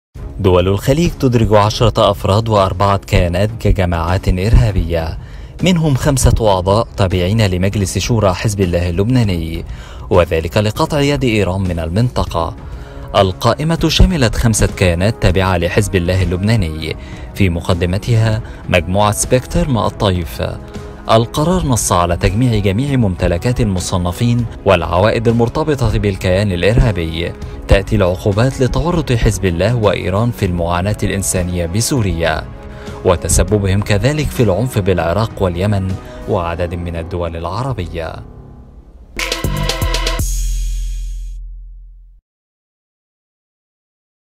دول الخليج تدرج 10 من حزب الله على قائمة الإرهاب
عنوان الفيديو | دول الخليج تدرج 10 من حزب الله على قائمة الإرهاب